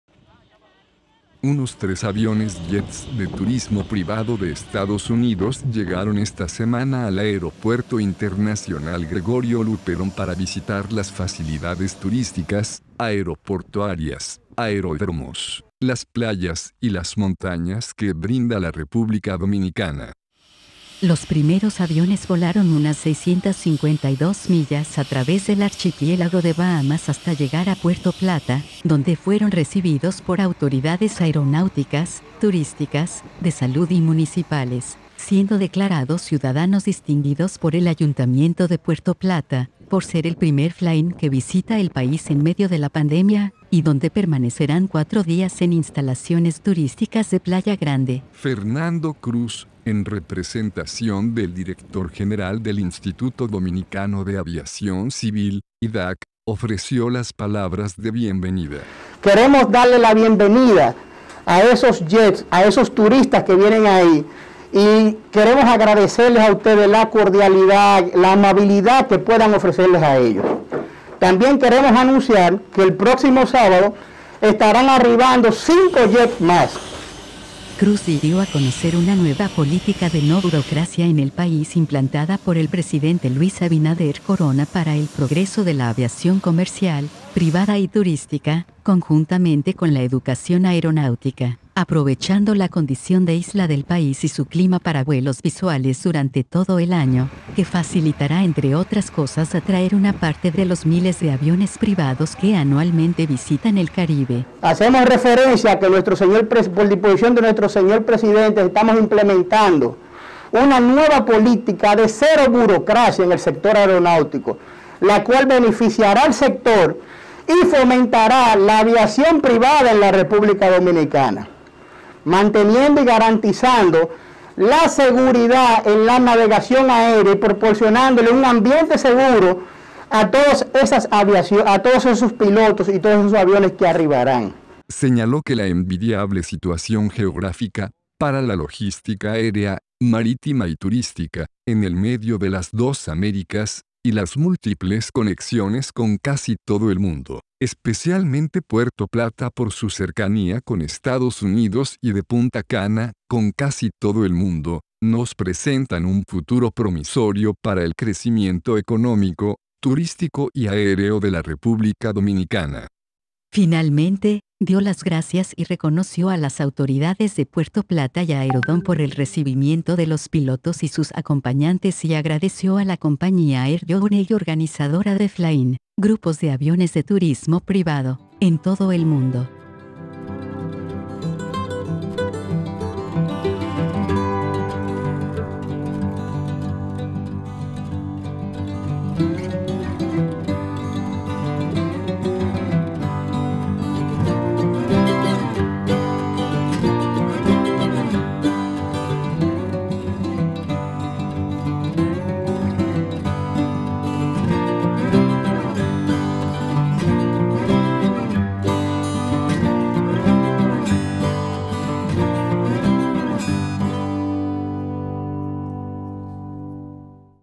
Turismo de aviación privada se reactiva con arribo aviones desde EE UU (IDAC)
Tres aviones Jets de turismo privado de Estados Unidos llegaron esta semana al Aeropuerto Internacional Gregorio Luperón para visitar las facilidades turísticas, aeroportuarias, aeródromos, las playas y las montañas que brinda la República Dominicana.

Los primeros aviones volaron unas 652 millas a través del archipiélago de Bahamas hasta llegar a Puerto Plata, donde fueron recibidos por autoridades aeronáuticas, turísticas, de salud y municipales, siendo declarados ciudadanos distinguidos por el Ayuntamiento de Puerto Plata, por ser el primer Fly-in que visita el país en medio de la pandemia, y donde permanecerán cuatro días en instalaciones turísticas de Playa Grande. Se estima recibir cinco aviones adicionales este próximo sábado 12 de diciembre.

Fernando Cruz, en representación del director general del Instituto Dominicano de Aviación Civil (IDAC), Román E. Caamaño, ofreció las palabras de bienvenida, señalando que el sistema de navegación aérea y aeroportuaria les garantiza una feliz estadía y que pueden contar con todas las facilidades de los aeropuertos y los 18 aeródromos para que conozcan los polos turísticos con los que cuenta el país y los que están por desarrollarse, como el de Bahía de Las Águilas.

Cruz dio a conocer una nueva política de no burocracia en el país implantada por el presidente Luis Abinader Corona para el progreso de la aviación comercial, privada y turística, conjuntamente con la educación aeronáutica, aprovechando la condición de isla del país y su clima para vuelos visuales durante todo el año, que facilitará entre otras cosas atraer una parte de los miles de aviones privados que anualmente visitan el Caribe.

Señaló que la envidiable situación geográfica para la logística aérea, marítima y turística, en el medio de las dos Américas, y las múltiples conexiones con casi todo el mundo, especialmente Puerto Plata por su cercanía con Estados Unidos y de Punta Cana con casi todo el mundo, nos presentan un futuro promisorio para el crecimiento económico, turístico y aéreo de la República Dominicana.

Finalmente, dio las gracias y reconoció a las autoridades de Puerto Plata y a AERODOM por el recibimiento de los pilotos y sus acompañantes y agradeció a la compañía AIR-YJOURNEY organizadora de Fly-in, grupos de aviones de turismo privado, en todo el mundo.